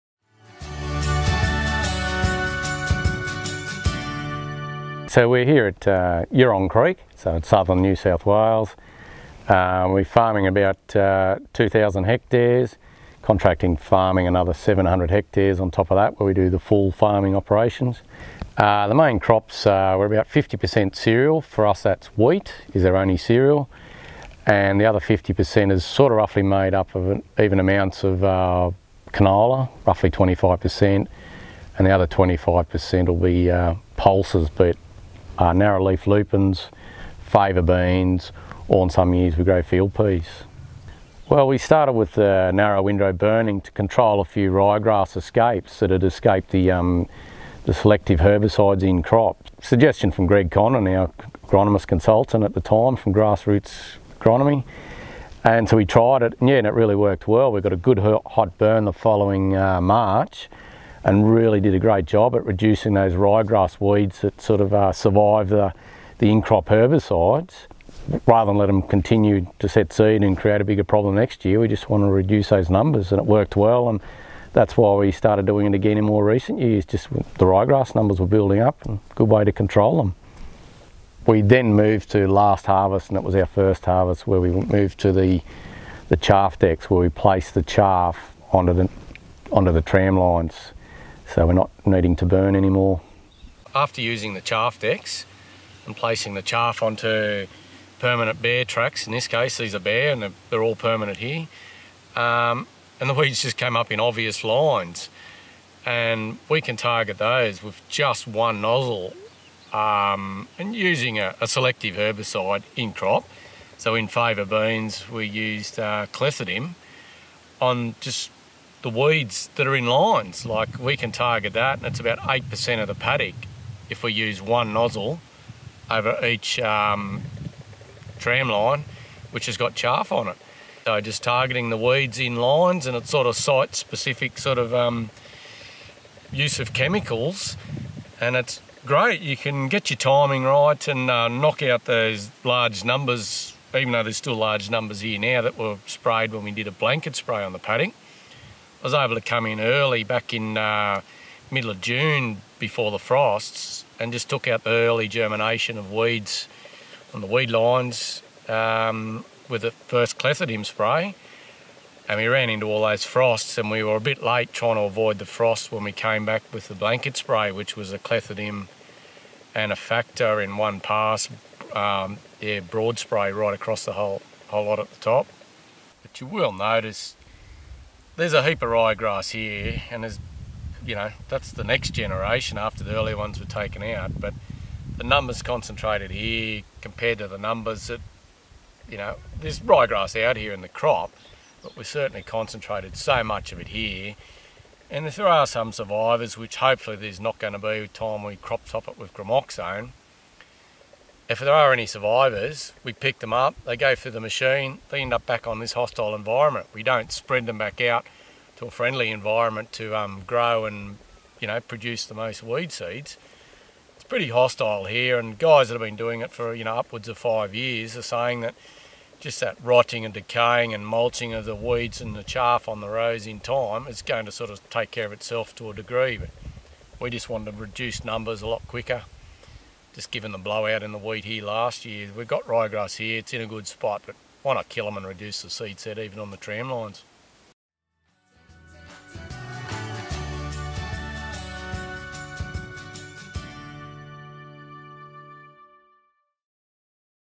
WeedSmart Tips Warwick Holding – Weed-lining
The knowledge that harvest weed seed control really works has fuelled the journey that Yerong Creek growers, Warwick and Di Holding, have taken to see the practice implemented on their farms, in every crop, every year. Site-specific weed-lining after harvesting using a chaff deck has helped reduce weed numbers quickly.